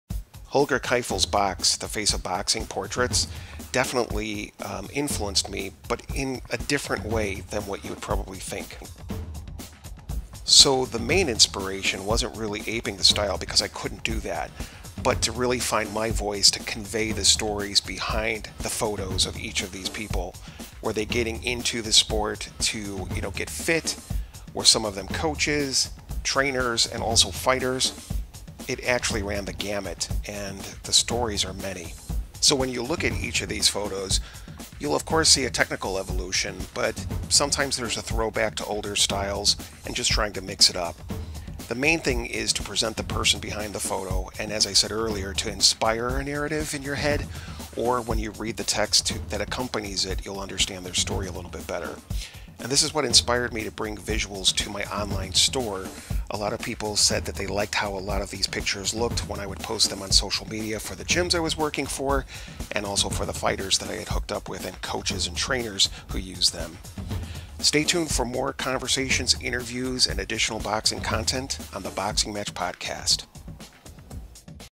Boxing Photography Inspirations: Holger Keifel "Box"...
Holger Keifel's photographs are next level badass and they continue to inspire me to find my photographic voice when capturing photos myself. Check out a glimpse of his book and some of my favorite boxing photographs.